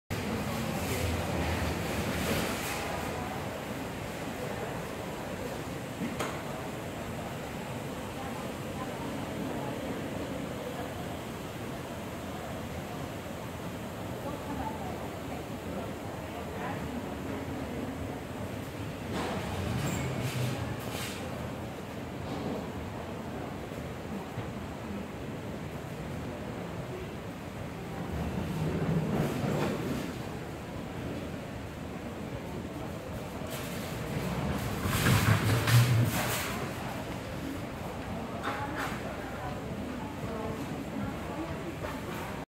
好市多中華店 "黯淡無光" 5分鐘之後恢復來電
緊急啟動發電機 僅供部分照明 電梯 手扶梯使用